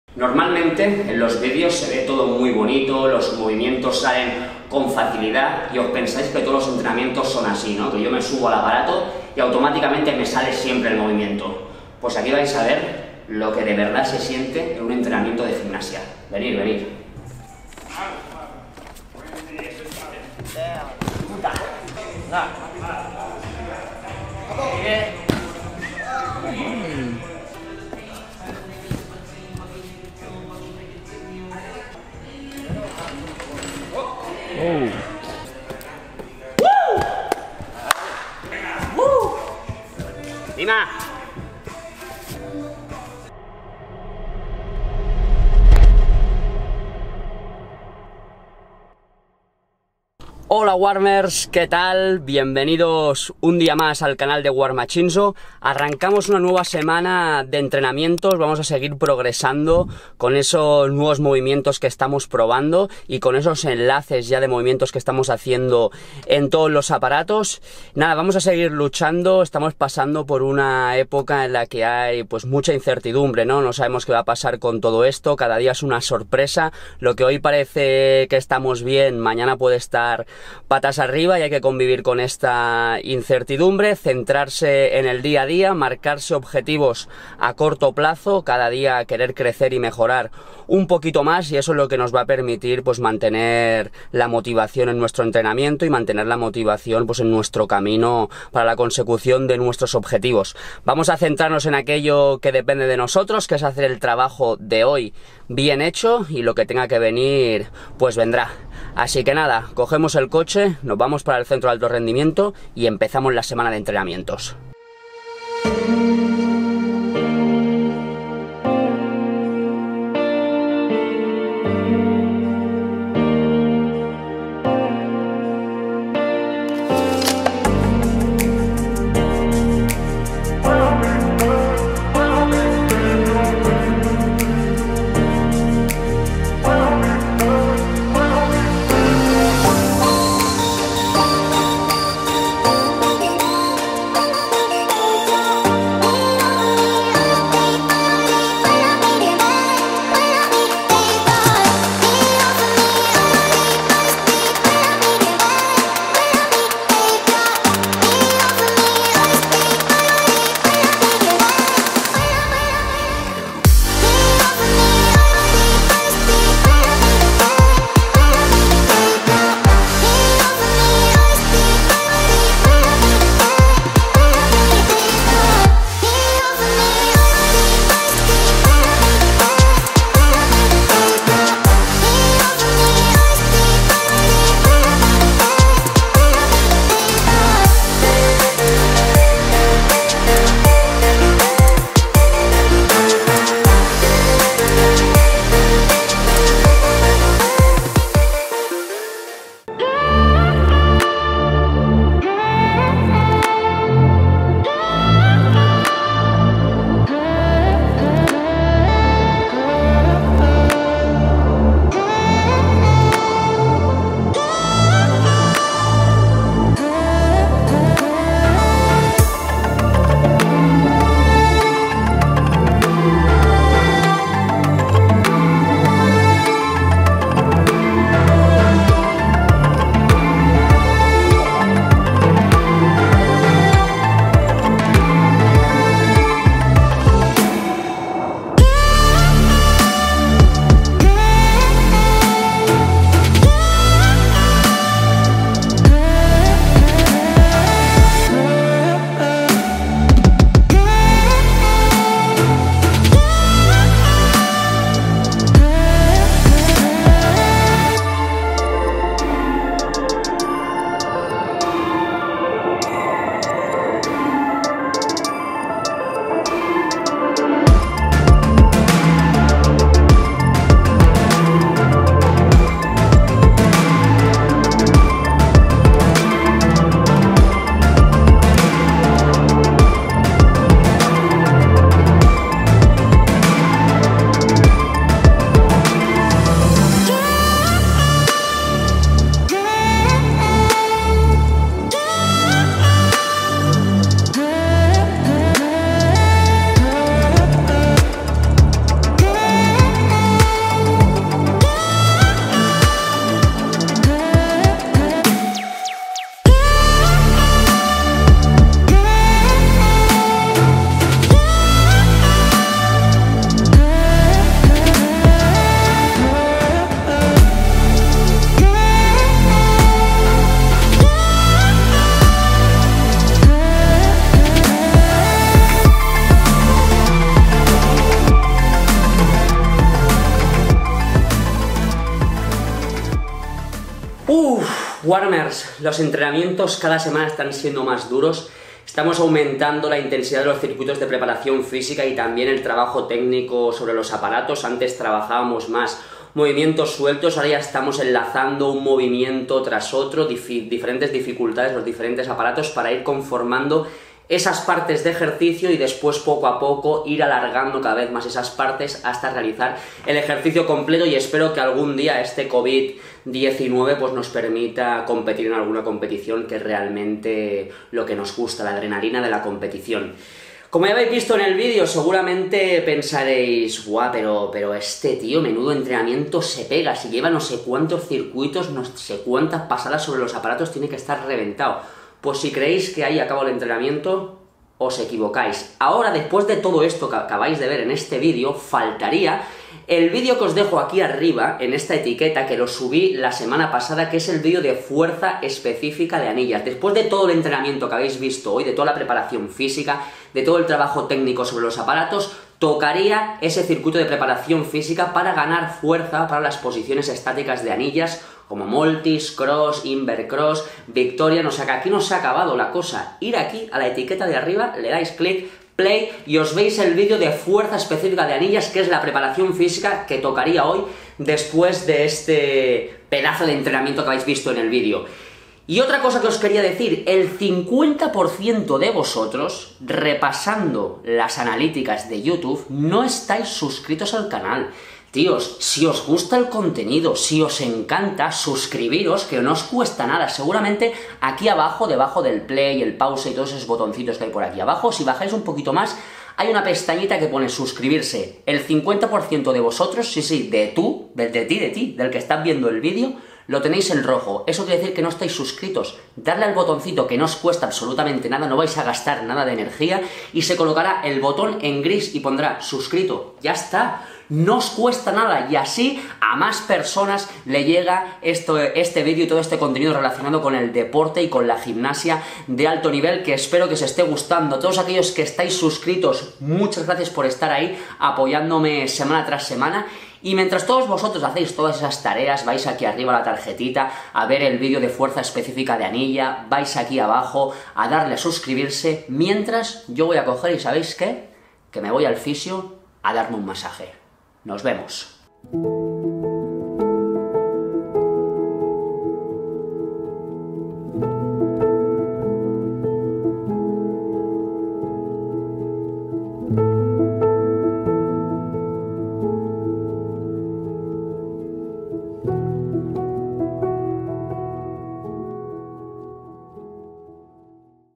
La RUTINA de ENTRENAMIENTO EXTREMA de un GIMNASTA🔥
Mira este vídeo y descubre cuál es el deporte más completo y exigente del mundo🔥
CANAL SECUNDARIO🎥

——-W A R M A X I N S O——-